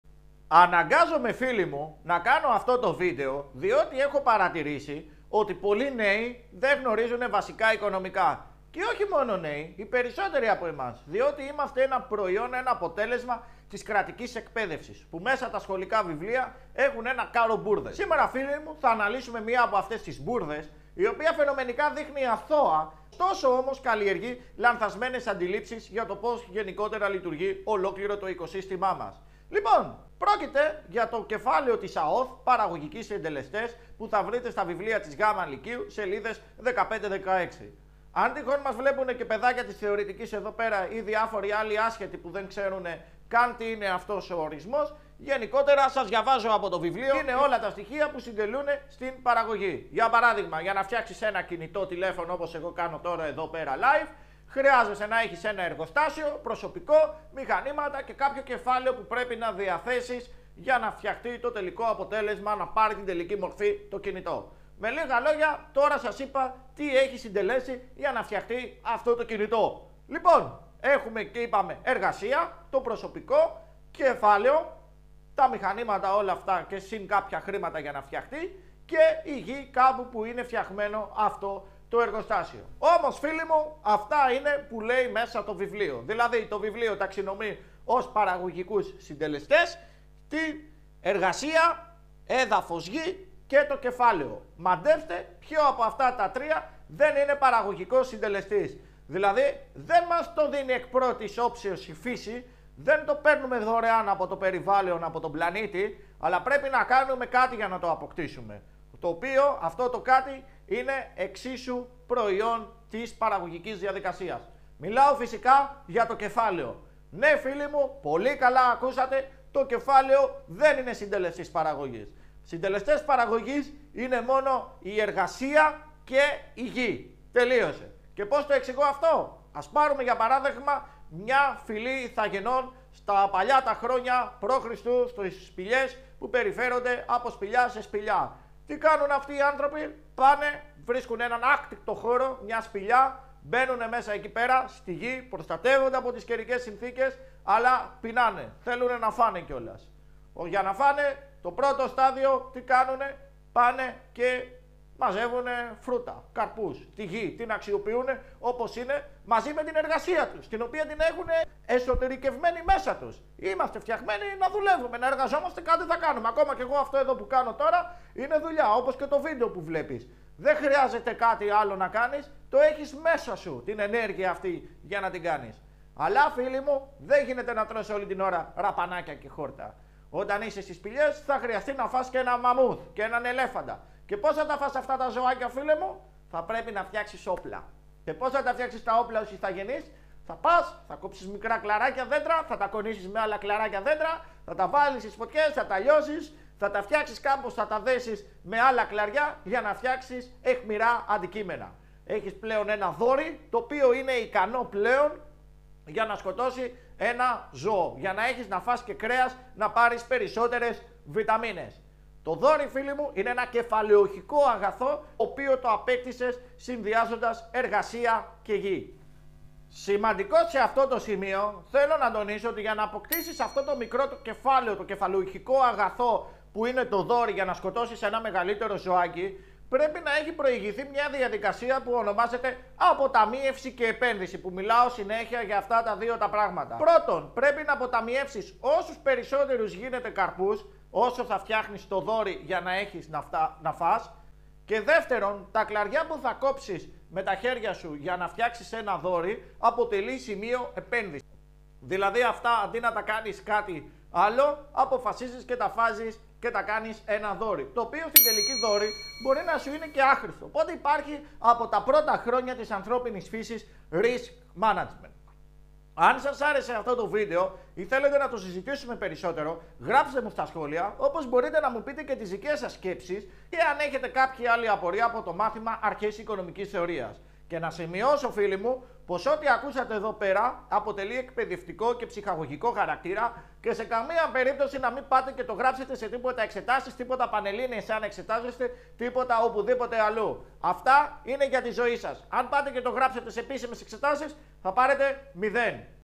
Αυτό Που Σου Είπαν Λάθος Στο Σχολείο Για Τους Παραγωγικούς Συντελεστές
Δυστυχώς το συγκεκριμένο βιβλίο έχει ορισμένα λαθη που οδηγούν σε λανθασμένες
νοοτροπίες το άτομο τόσο στη κοινωνική ζωή όσο και στην επιχειρηματική δραστηριότητα!

Θα τα αναλυσουμε ολα μαζι παρέα, ελπίζω να το απολαυσετε!

Συνοπτικά το βιβλιο λέει οτι παραγωγικοι συντελεστές είναι:
α) Εργασία
β) Γη
γ) Κεφάλαιο

αλλά στη πραγματικότητα το κεφαλαιο προέρχονται από την εργασία και τη γη, που προϋπάρχει !
Το κεφάλαιο είναι προϊον παραγωγής, το οποιο επαναχρησιμοποείται για την παρασκευή νέου προϊόντος